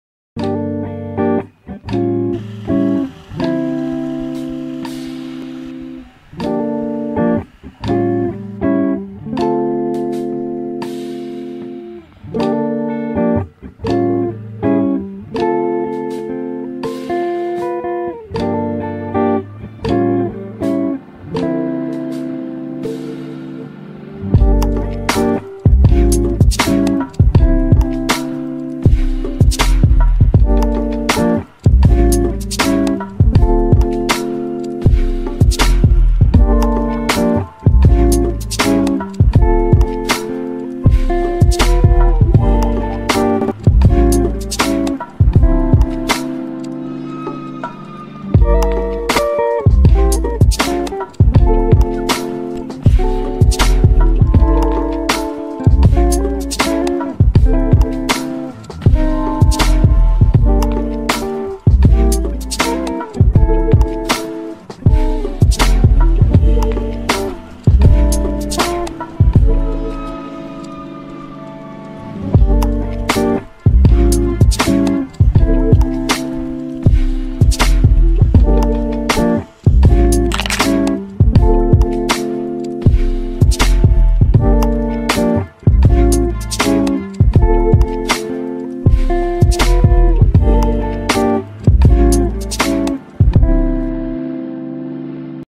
mountain chill training #2
went with Raul on the mountains again and i recorded few clips.
not my best edit so far,but i got a better pc,so i just tryed it with some chill clips recorded from there,so this video is just a test,don't mind at it.